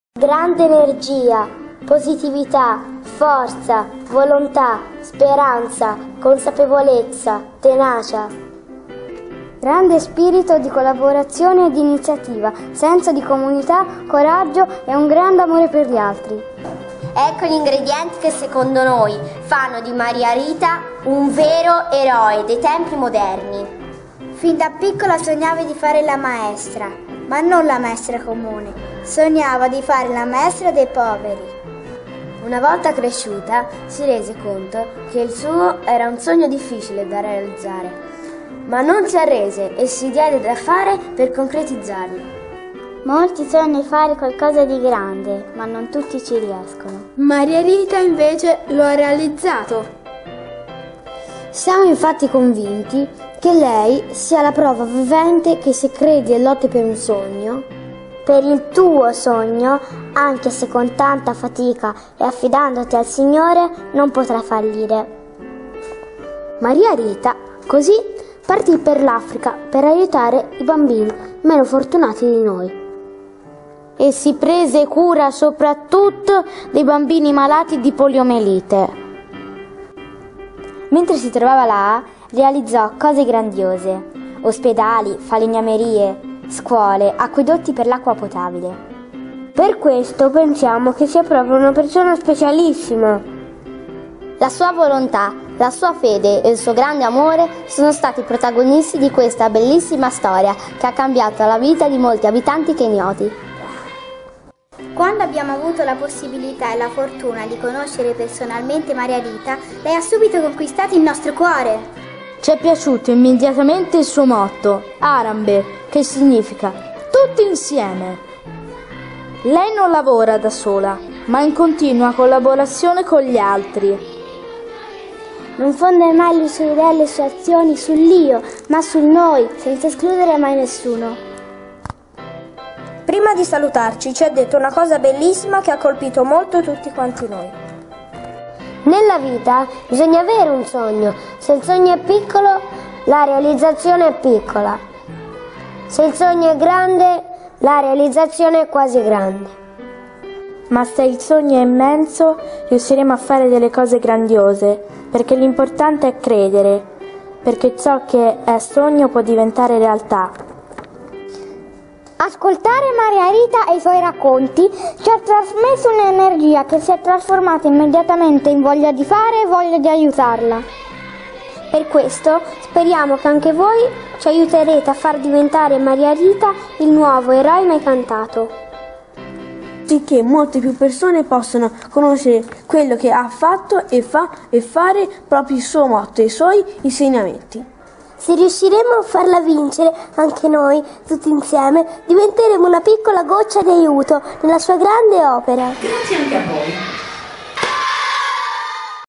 Maria Rita Tarquini - Messa In Spazio (completo)
Messa in spazio per l'EroeMaiCantato candidato dalla classe IV della Scuola Elementare "Santa Caterina" di Pisa.